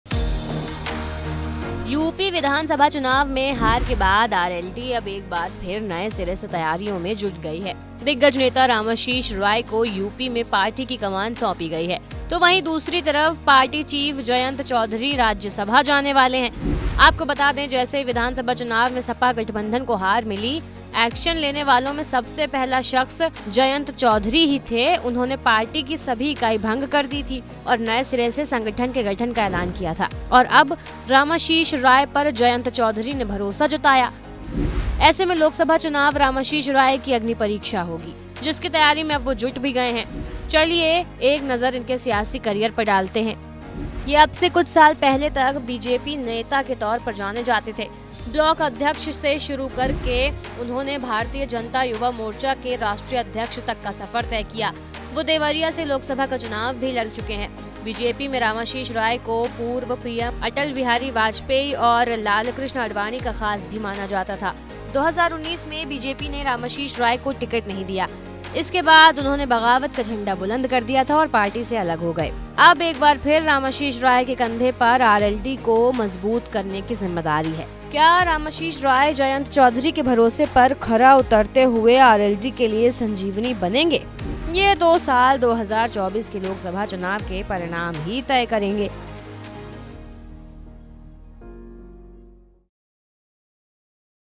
Jayant Choudhary ने Ramashish Rai को बनाया प्रदेश अध्यक्ष, RLD को मिलेगी संजीवनी ?
Ramashish Rai को Jayant Choudhary ने  RLD का प्रदेश अध्यक्ष की कमान सौंपी है और वो खुद राज्यसभा जाने की तैयारी कर रहे हैं. अब देखना होगा कि रामाशीष राय आरएलडी के लिए संजीवनी साबित होते हैं या नहीं.